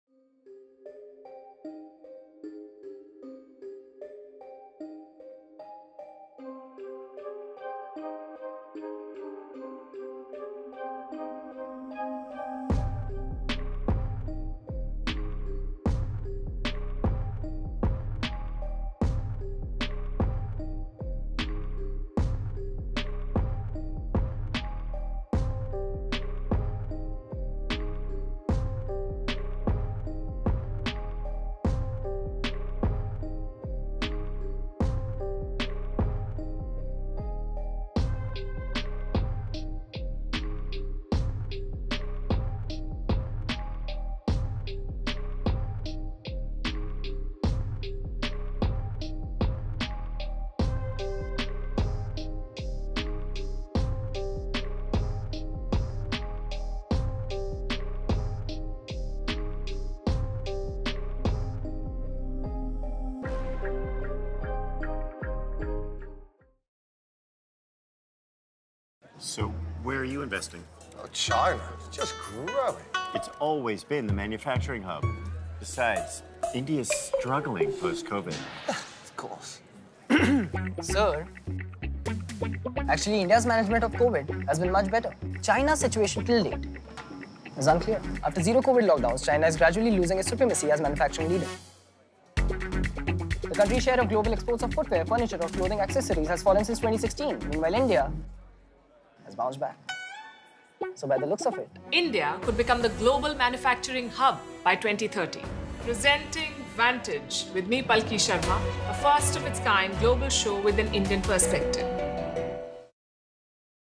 Up In Smoke: Backlash Over Italy's Proposed Outdoor Smoking Ban
The Italian Minister’s proposal to extend a ban smoking including the outdoor areas is facing scrutiny from the government. Italy’s ministers fume over the proposal and call the move “exaggerated”.